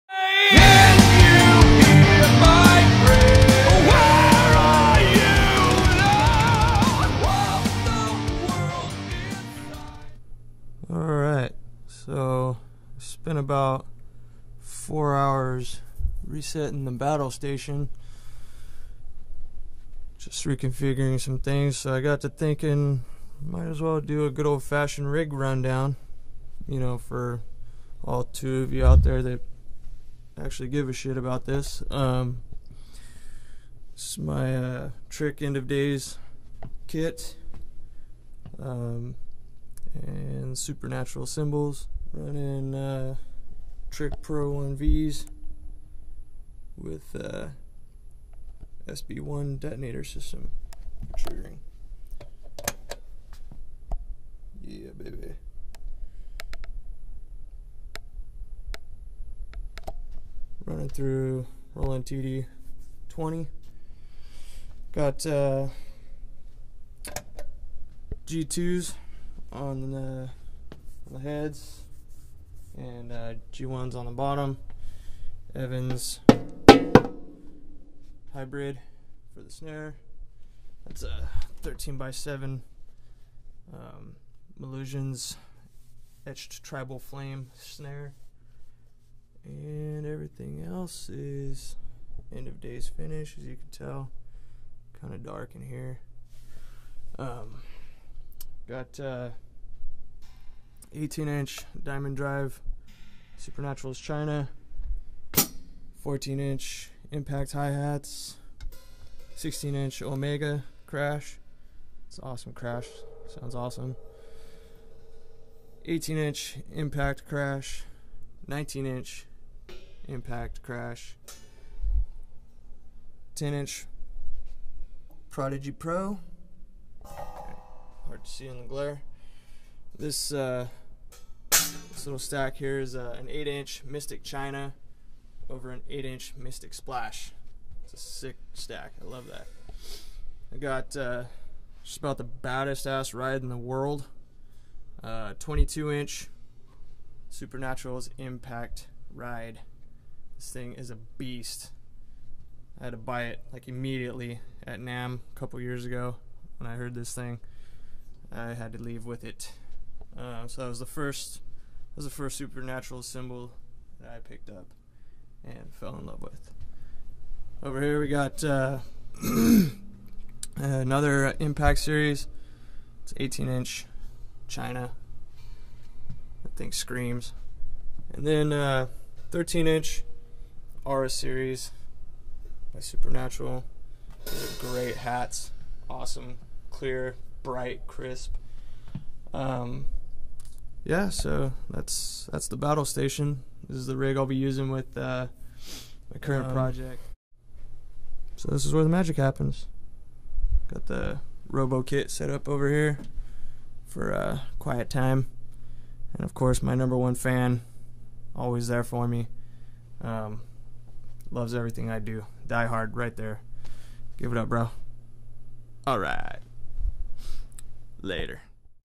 Rig Rundown
Just going over my gear and current setup.